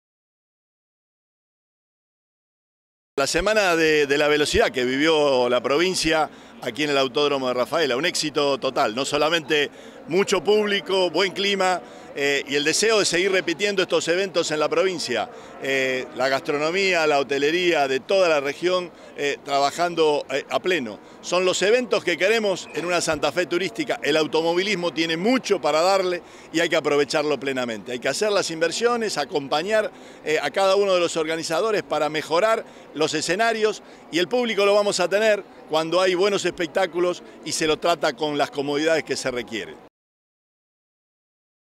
PEROTTI PARTICIPÓ DE LA CLAUSURA DE LA SEMANA DE LA VELOCIDAD EN RAFAELA
El evento automovilístico, que comenzó el 24 de julio, cerró hoy con las tres finales del Top Race. “Estos son los eventos que queremos en una Santa Fe turística”, sostuvo Perotti. Mas Información